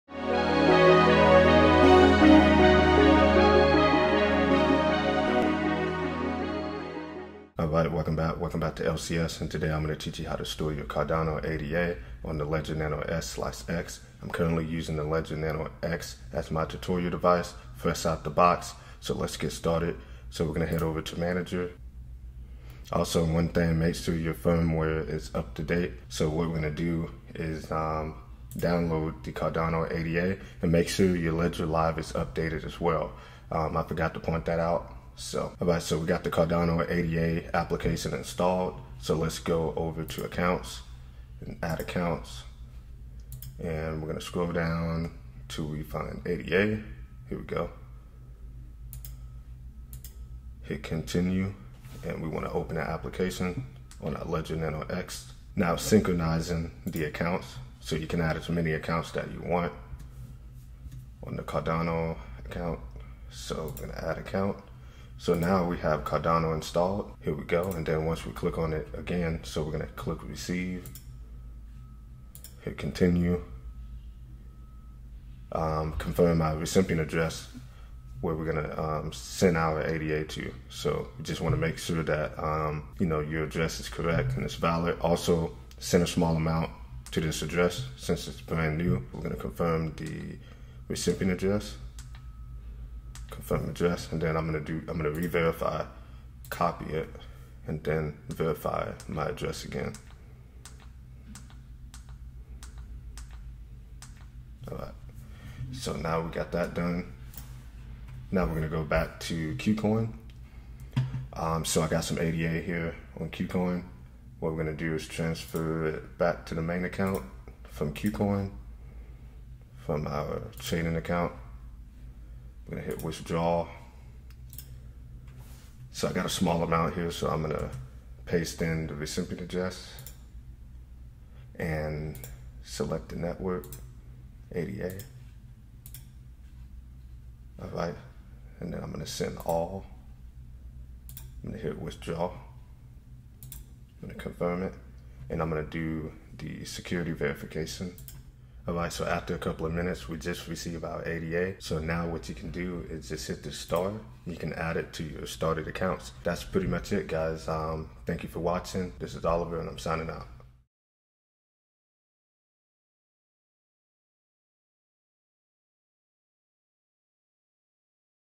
🔐 How To Store Your Cardano (ADA) On Ledger Live - Step-by-step Tutorial!
Cardano is a third-generation smart contract platform, similar to Ethereum, focusing on security through a layered architecture. We're doing a step-by-step tutorial on storing the digital asset Cardano 'ADA' onto Ledger Live and the hardware cold storage wallet 'Ledger.'

This Ledger Live process for Cardano 'ADA' is compatible with the 'Ledger Nano S, Ledger Nano S Plus, and Ledger Nano X.'

Chapters:
0:00 Installing Cardano (ADA) Application
0:38 Adding Cardano (ADA) Accounts
1:09 Receiving Cardano (ADA) Addresses
1:51 Withdrawing Cardano (ADA) Exchanges
2:28 Storing Cardano (ADA) Ledger Live